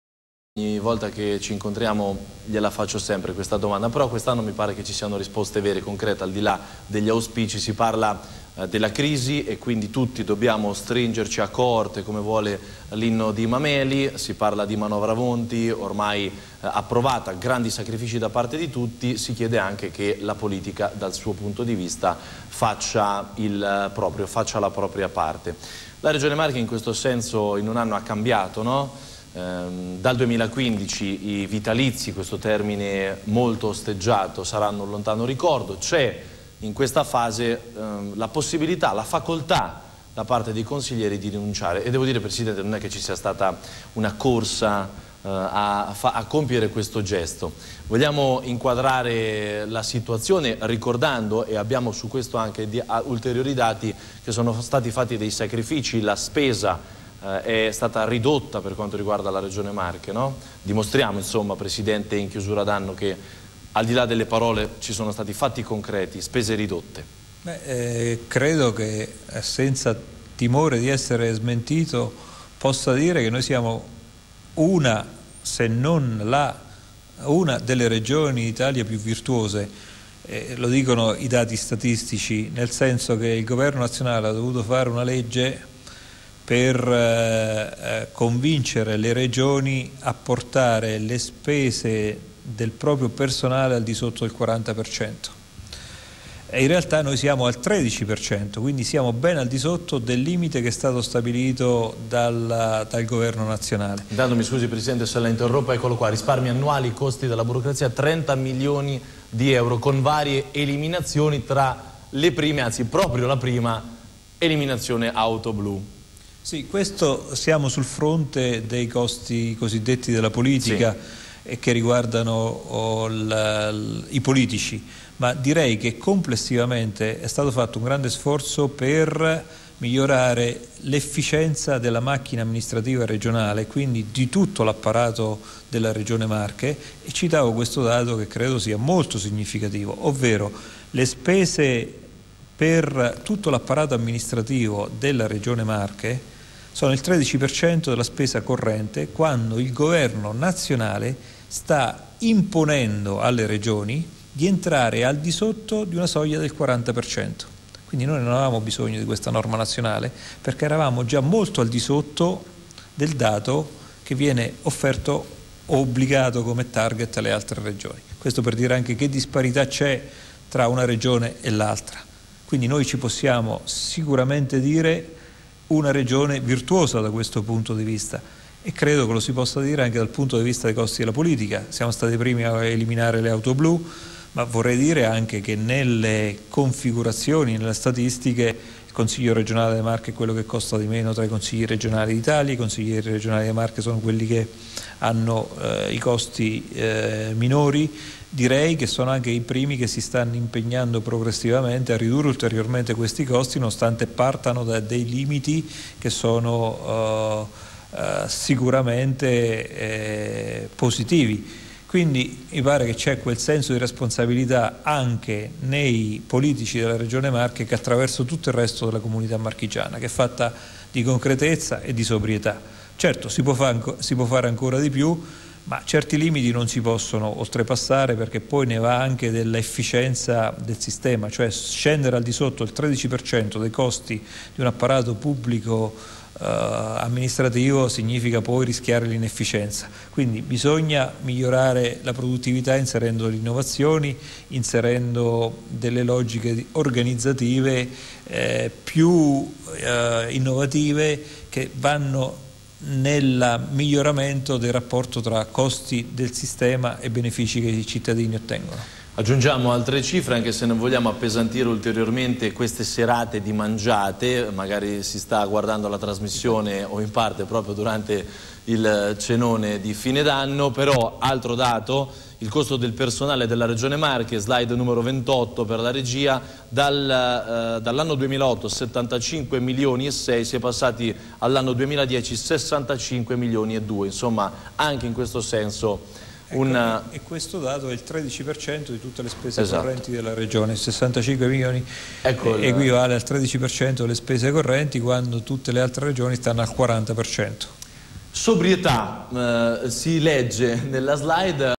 Costi della politica e della macchina amministrativa regionale (Spacca-Etv).wmv
Anche su costi della macchina amministrativa siamo una delle Regioni più virtuose, grazie al grande sforzo per ottimizzare le spese e migliorare l'apparato amministrativo della Regione, che pesa il 13% della spesa corrente, mentre nelle altre Regioni il Governo Monti ha chiesto di scendere al di sotto del 40%...